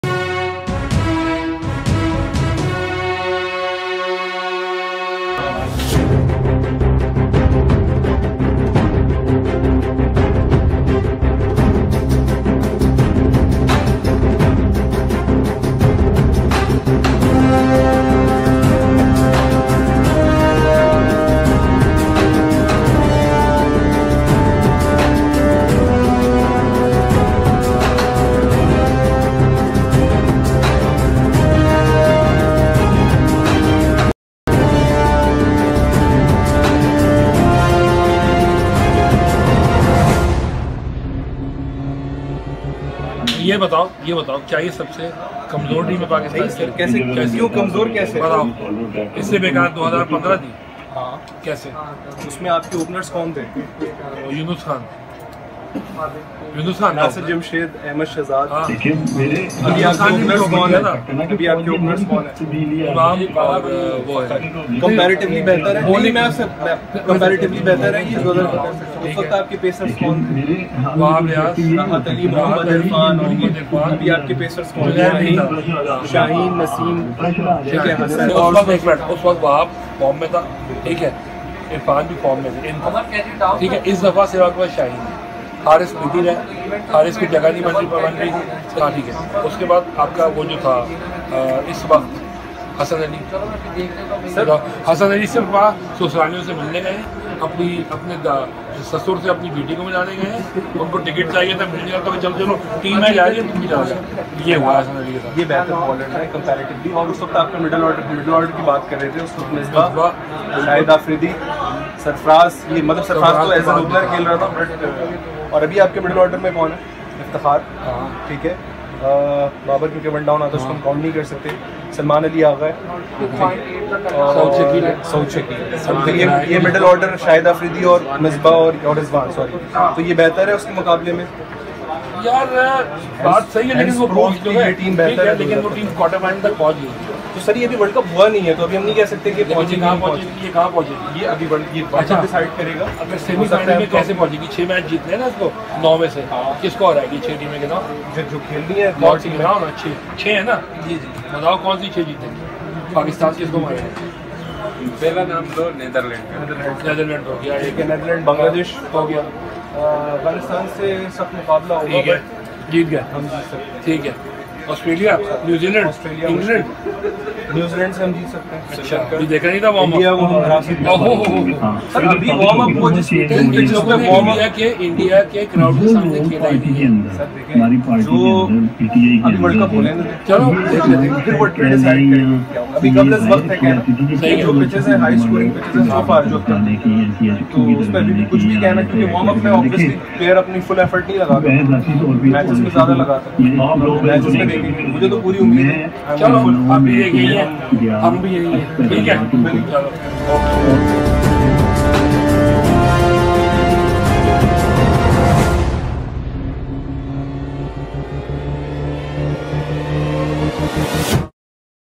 Is this Pakistan's worst World Cup squad?misbahulhaq
Candid discussion with my brilliant colleague Rubais Ali Shahid on the composition of Pakistan team. Rubais has made me believe that there is still some hope expected from the next generation. The current squad is a lot better than the one from 2015, he believes that.